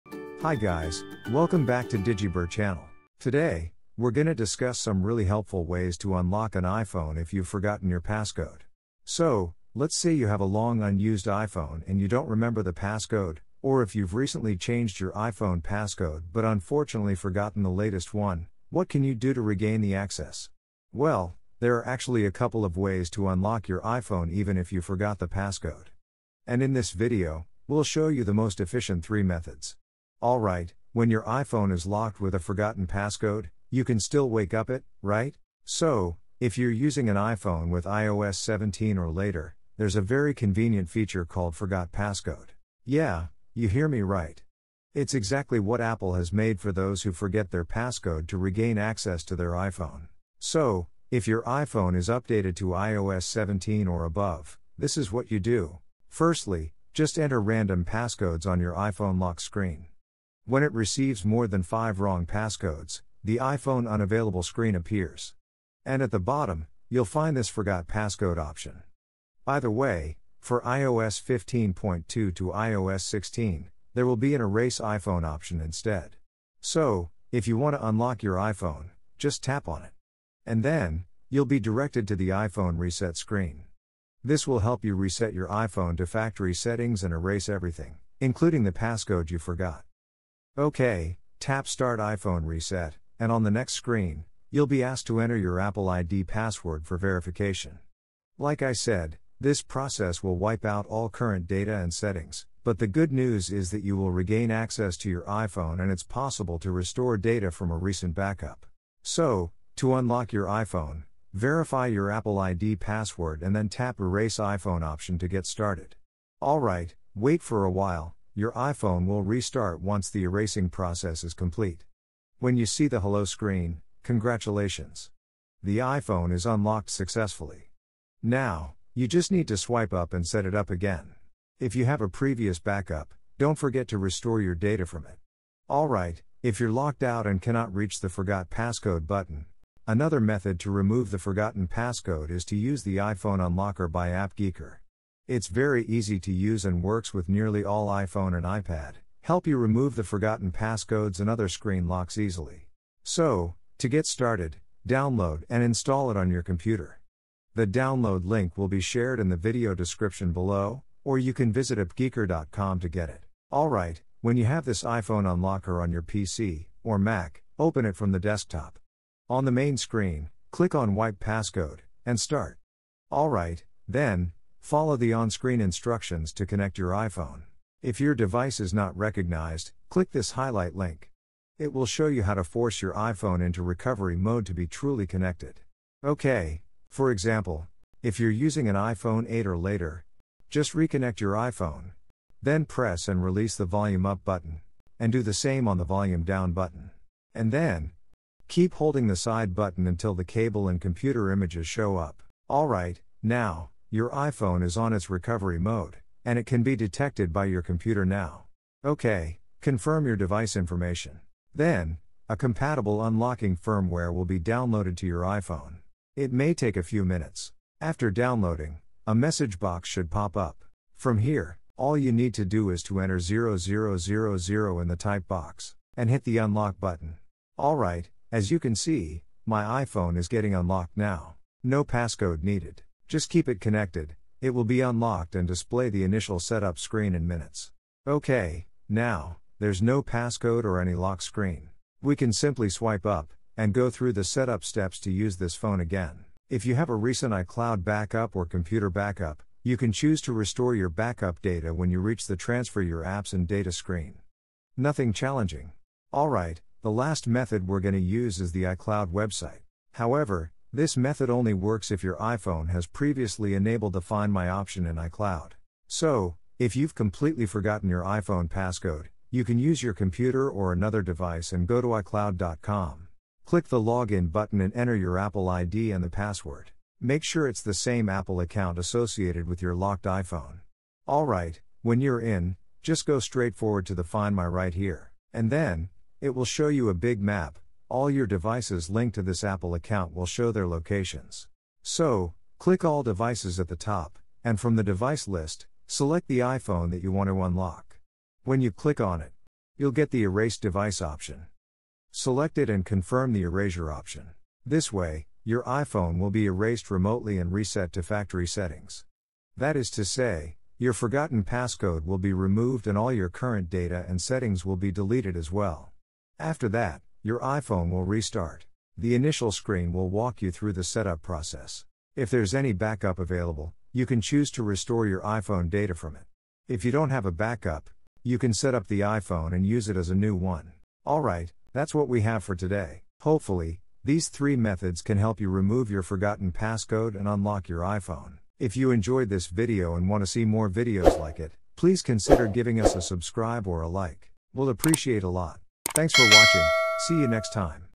Forgot iPhone Passcode – 3 Efficient Ways to Unlock iPhone Passcode
To remove a forgotten passcode on iPhone, this iPhone Unlocker software does an excellent job

Timestamp:
00:00 Intro
00:36 Method 1. Forgot Passcode in iOS 17 or later
02:20 Method 2. iPhone Unlocker by AppGeeker
04:17 Method 3. Erase iPhone in iCloud Find My

It’s not the end of the world if you’ve forgotten your iPhone passcode. This video explores 3 proven ways to unlock an iPhone without passcode:

1. The Forgot Passcode option in iOS 17 or later allows iPhone users to erase the forgotten passcode along with everything else. By verifying your Apple ID password, you can reset the iPhone and regain access.

2. If the Forgot Passcode option doesn’t show up, another efficient tool iPhone Unlocker can be of use. With just a few simple clicks, it enables users to wipe passcode and unlock an iPhone or iPad in minutes.

3. When the Find My is previously enabled in iCloud, it can remotely erase all current data and settings on the locked iPhone, including the lost passcode.

Note that all these solutions will erase your iPhone and wipe everything on it. After the forgotten passcode is removed, you can set up your iPhone again from the beginning. If there are any backups available, try restoring your iPhone from a recent backup to prevent data loss.